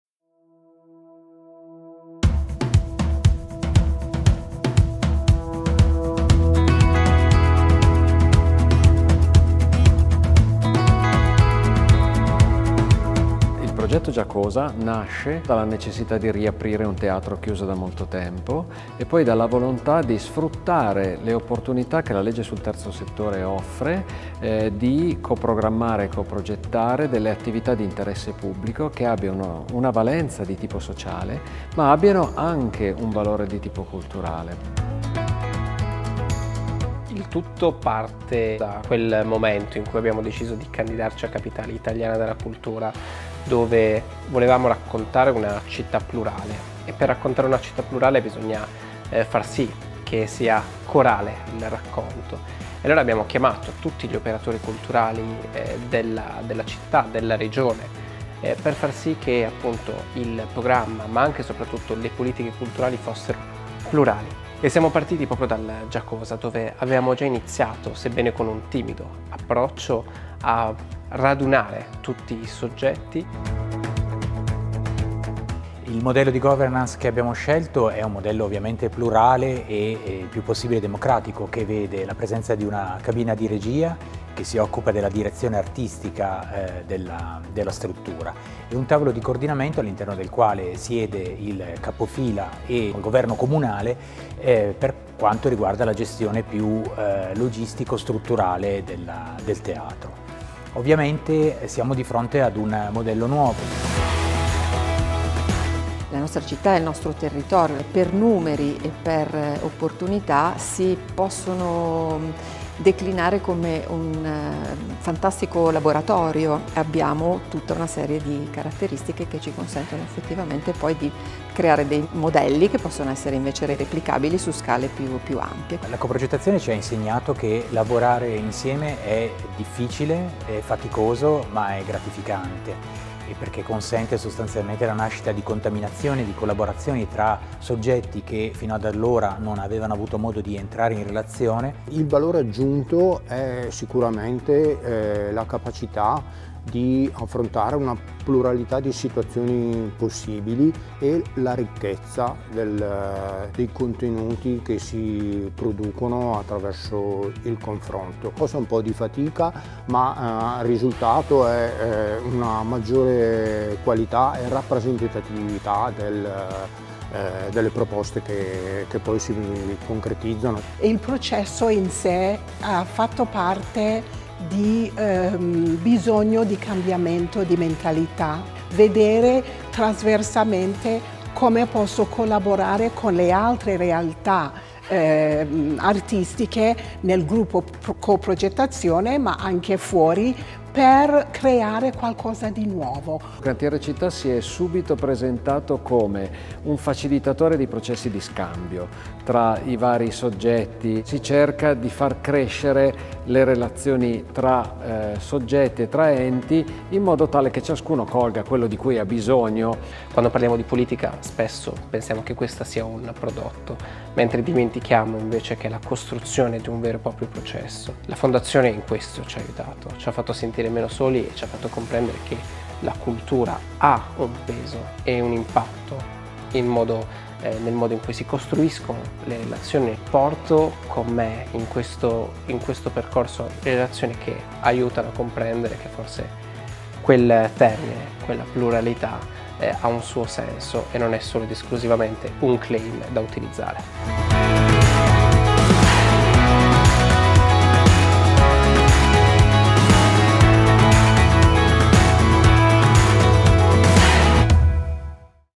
Cantiere Città II edizione - Aosta
𝗖𝗮𝗻𝘁𝗶𝗲𝗿𝗲𝗖𝗶𝘁𝘁𝗮̀ è il percorso di capacity building ideato da Ministero della Cultura e dalla Fondazione per 𝗽𝗿𝗼𝗺𝘂𝗼𝘃𝗲𝗿𝗲 𝗲 𝘃𝗮𝗹𝗼𝗿𝗶𝘇𝘇𝗮𝗿𝗲 𝗶 𝗽𝗿𝗼𝗴𝗲𝘁𝘁𝗶 presentati dalle città finaliste del programma 𝘊𝘢𝘱𝘪𝘵𝘢𝘭𝘦 𝘪𝘵𝘢𝘭𝘪𝘢𝘯𝘢 𝘥𝘦𝘭𝘭𝘢 𝘤𝘶𝘭𝘵𝘶𝘳𝘢 2025 cidc2025.

Un percorso individuale è riservato ad ogni città finalista attraverso tre 𝘄𝗼𝗿𝗸𝘀𝗵𝗼𝗽 che, tenendo in conto la specificità di ciascuna realtà territoriale, possono sostenere le città candidate nella attuazione della loro 𝗽𝗿𝗼𝗴𝗿𝗮𝗺𝗺𝗮𝘇𝗶𝗼𝗻𝗲 𝗰𝘂𝗹𝘁𝘂𝗿𝗮𝗹𝗲.

Ad Aosta si è lavorato sulla co-progettazione per la riapertura, la governance e la programmazione della 𝘀𝘁𝗮𝗴𝗶𝗼𝗻𝗲 𝘁𝗲𝗮𝘁𝗿𝗮𝗹𝗲 𝟮𝟬𝟮𝟯/𝟮𝟬𝟮𝟰 𝗱𝗲𝗹 𝗧𝗲𝗮𝘁𝗿𝗼 𝗚𝗶𝗮𝗰𝗼𝘀𝗮. L'obiettivo è stato di sviluppare un nuovo modello di gestione partecipato che coinvolga attivamente la comunità locale, promuovendo l'integrazione tra la città e il territorio. Numerose associazioni ed enti del Terzo settore aostani, attivi nel settore degli spettacoli dal vivo, sono stati coinvolti, tra cui 𝗔𝗿𝗰𝗶 𝗩𝗮𝗹𝗹𝗲 𝗱'𝗔𝗼𝘀𝘁𝗮, 𝗔𝗶𝗮𝗰𝗲 𝗩𝗮𝗹𝗹𝗲 𝗗'𝗔𝗼𝘀𝘁𝗮, 𝗟𝗼𝗻𝗴 𝗡𝗲𝗰𝗸 / 𝗖𝗮𝗰𝘁𝘂𝘀 𝗙𝗶𝗹𝗺 𝗙𝗲𝘀𝘁𝗶𝘃𝗮𝗹, 𝗧𝗿𝗮𝗶𝘁 𝗱'𝗨𝗻𝗶𝗼𝗻, 𝗟𝗮 𝗠𝗲𝗹𝗮𝗴𝗿𝗮𝗻𝗮.